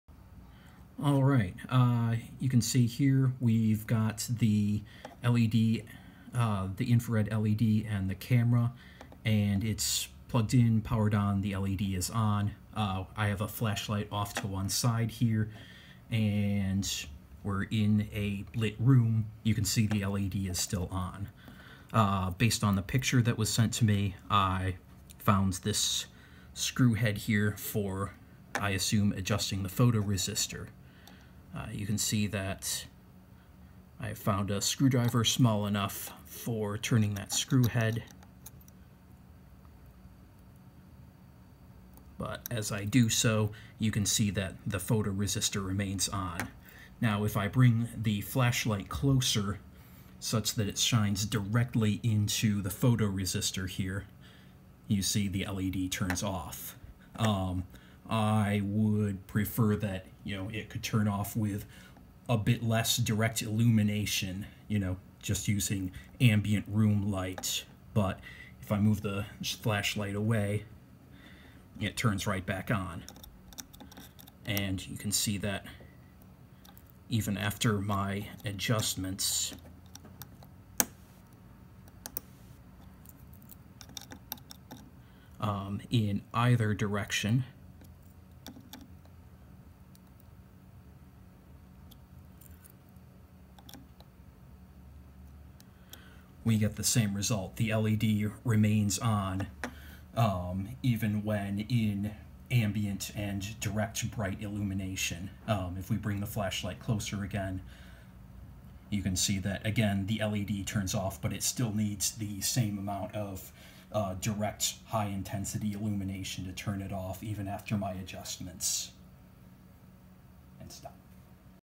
Waveshare IR LED + photoresistor test
Here I attempt adjusting the sensitivity of the photoresistor, but my adjustments do not seem to change it's sensitivity to light in the room